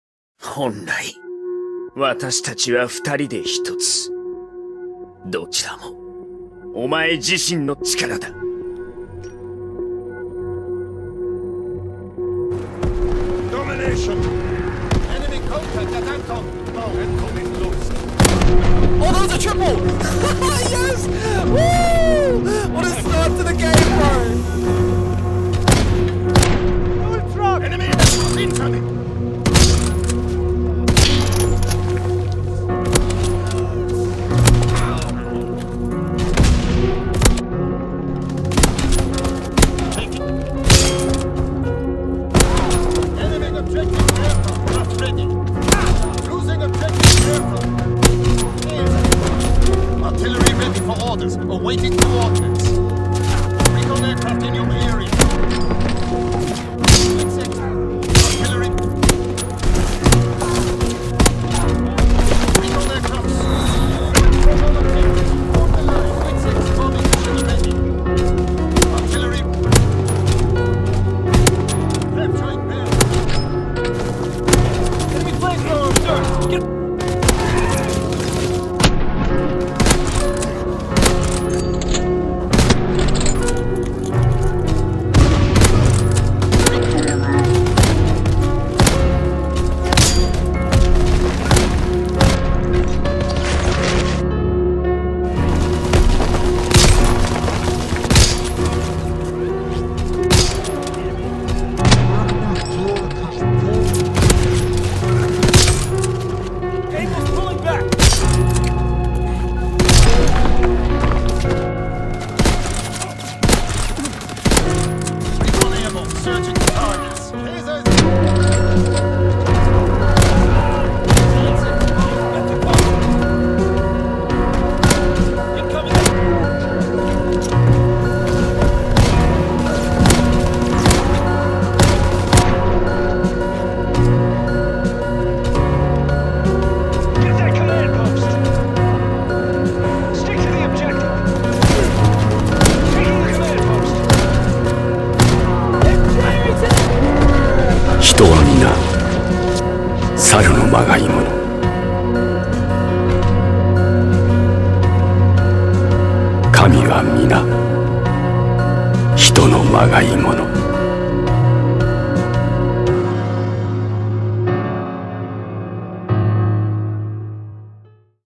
Chai - WW2 Montage 2
made this quicker than expected. opener & closer for my tage were in within 48 hours. be on the lookout for that soon. 24 hour charity stream will be 3rd of July. hope you all enjoy.

Be sure to follow me on all my platforms to stay up to date with what I'm doing regarding streams & videos.